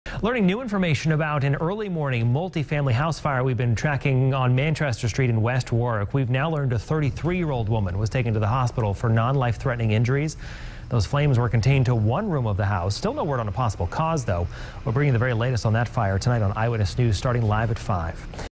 West Warwick Fire Sends Woman to the Hospital
The flames were contained to one room inside the home on Manchester Street.